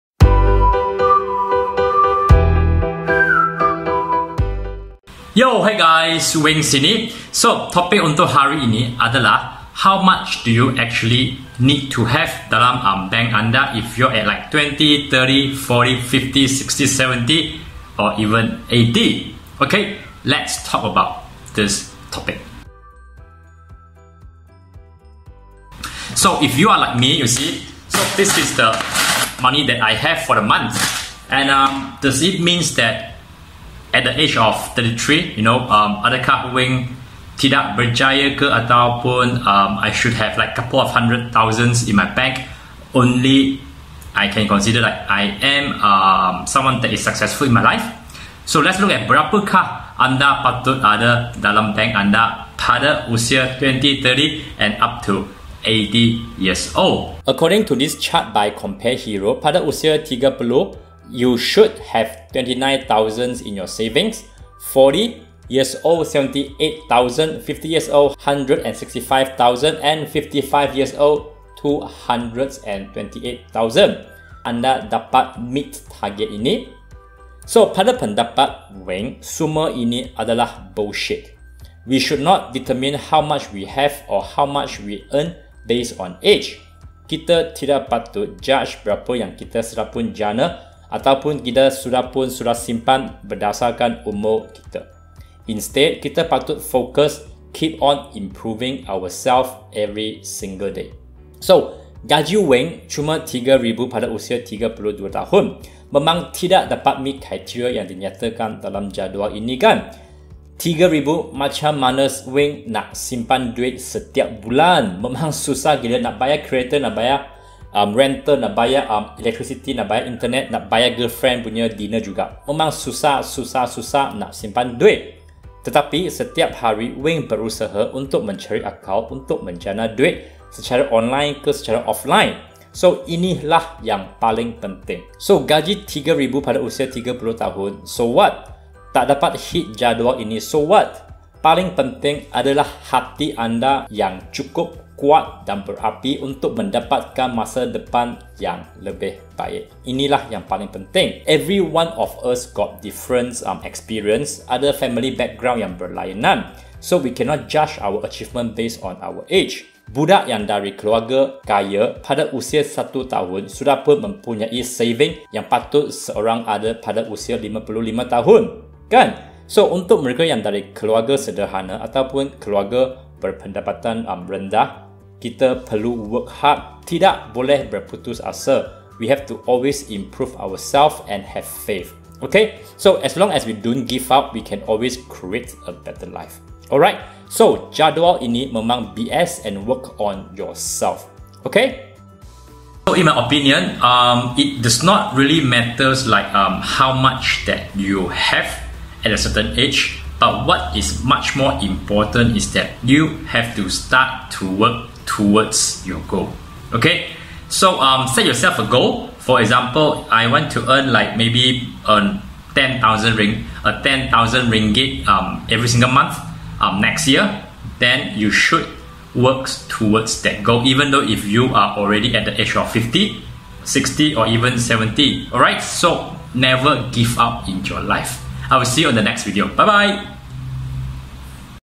INILAH DUIT YANG ANDA PERLU ADA PADA USIA 30,40,50 ,Usia 30 RM 30,000 Usia 40 RM78,000
Pada usia 30 you should have 29k in your saving
40 78k
50 165k
55 228k
So , adakah anda meet target ini?
Pada pendapat Weng semua ini adalah bullshit .
We should not determine how much we have or we earn based on age.
Kita tidak patut judge berapa yang kita jana , kita dah simpan dengan berdasarkan umur kita.
Instead kita patut focus kat improving ourselves every single day .
Gaji Weng Cuma 3RM pada usia 32 tahun . memang tidak dapat meet criteria yang dinyatakan dalam jadual ini .
Tetapi setiap hari saya , berusaha untuk mencari akal untuk menjana duit . Inilah yang paling penting.
Gaji Rm 3000 pada usia 32 tahun so what?tak dapat hit jadual ini so what?
Paling penting adalah hati berjuang untuk masa depan kita.
Setiap kita ada experience yang berlainan , ada family background yang berlainan .
So , we cannot judge our achievement based on our age.
Budak dari keluarga kaya , pada usia 1 sudah pun mempunyai saving yang patut ada pada usia 55 .
Untuk mereka yang dari keluarga sederhana , atau keluarga berpendatan rendah , kita perlu work hard , tidak berputus asa , always improve ourself , and have faith .
As long as we don’t give up , we can always create a better life .
Alright , so , jadual ini memang BS , work on your self.